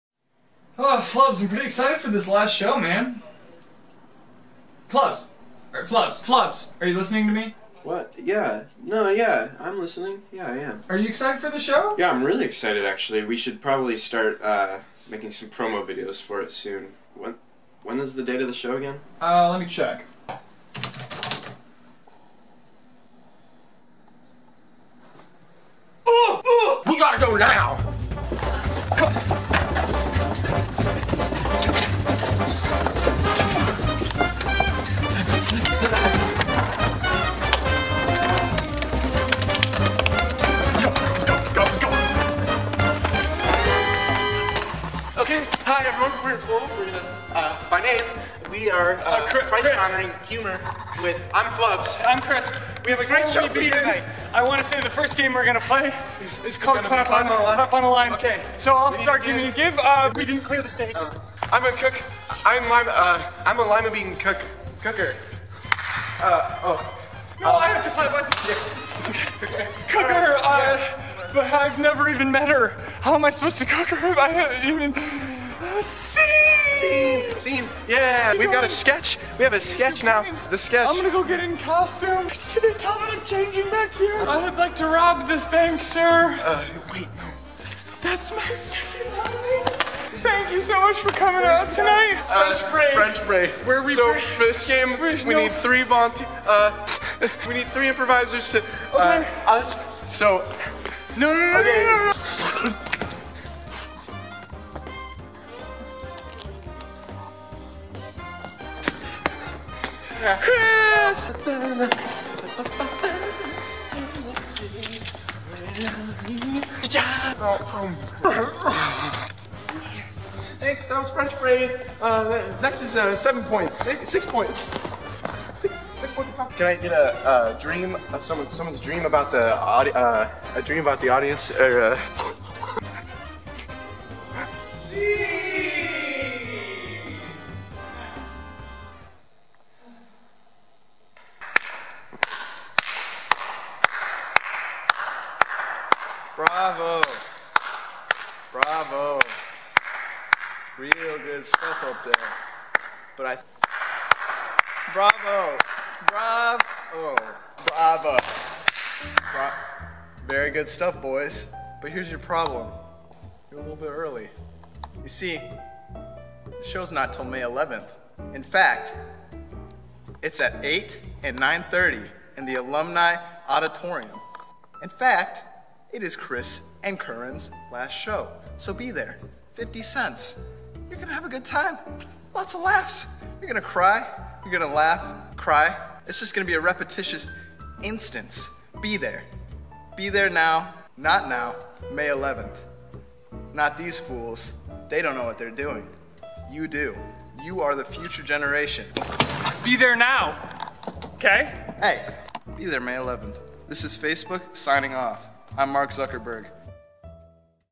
LAST [i] SHOW
[iNfoRmaL]'s LAST SHOW is this Saturday, May 11th!! At 8, and 9:30, come see Chris and Kern's FINAL show. It's gonna be a blast and a half.